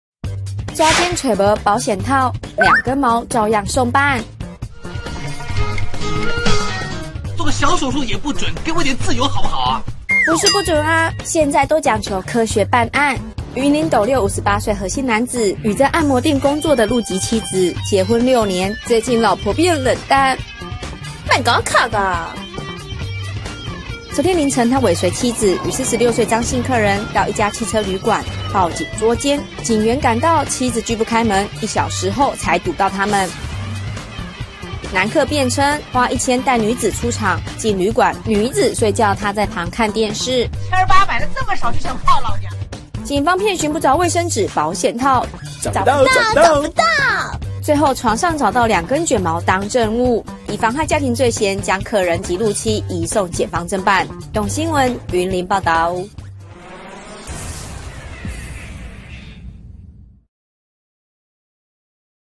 抓姦找嘸保險套 2根毛照樣送辦--蘋果日報 20140810
【寶智華╱雲林報導】雲林一名按摩女，昨凌晨跟客人到摩鐵開房間，丈夫尾隨報警捉姦，警方敲門但他們拒開門，直到30分鐘後要離去才被警方攔下，警方進房後在床上找到2根毛髮當證物，丈夫提告，2人雖否認，但仍被依妨害家庭罪嫌送辦。
何女在警局說：「我什麼也沒做，是被老公栽贓，把我銬著，感覺像犯人。」
警方調查，何姓按摩女（49歲）跟58歲的丈夫結婚6年，近日在一家連鎖護膚按摩店上班，因妻子對他態度冷淡，懷疑妻子外遇，昨凌晨等在按摩店外，發現妻子下班後被人載走，就一路尾隨到汽車旅館，凌晨4時25分報警。

抓姦找嘸保險套 2根毛照樣送辦
做個小手術也不准
給我一點自由好不好啊
不是不准啊
現在都講求科學辦案
雲林斗六58歲何姓男子
與在按摩店工作的陸籍妻子
結婚6年
最近老婆變冷淡
昨天凌晨
他尾隨妻子與46歲張姓客人
到一家汽車旅館
報警抓姦
警員趕到
妻子拒不開門
1小時後
才堵到他們
男客辯稱花1千
帶女子出場
進旅館女子睡覺
他在旁看電視
七二八百的就想泡老娘
警方遍尋不著衛生紙 保險套
最後床上找到2根捲毛
當證物
以妨害家庭罪嫌將
客人及陸妻移送檢方偵辦
動新聞 雲林報導